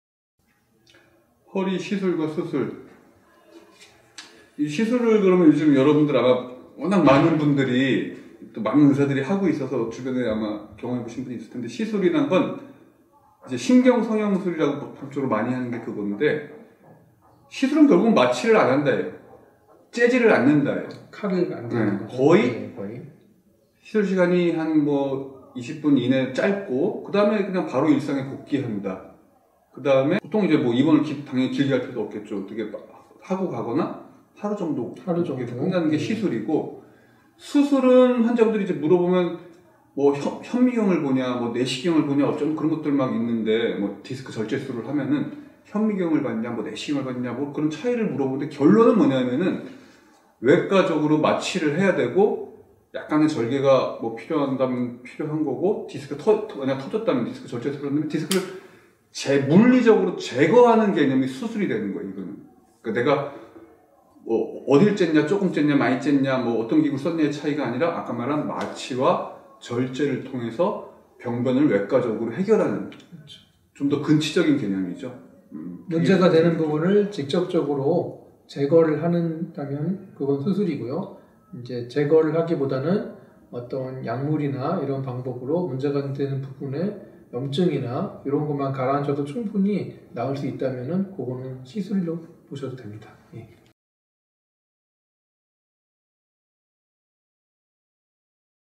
시술과 수술의 차이점, 광명새움병원
광명새움병원에서 시술과 수술의 차이점을 간단하게 설명해드립니다.👍
(정형외과 전문의 이상준 원장 // 마취통증의학과 전문의 박청훈 원장)

✅진료문의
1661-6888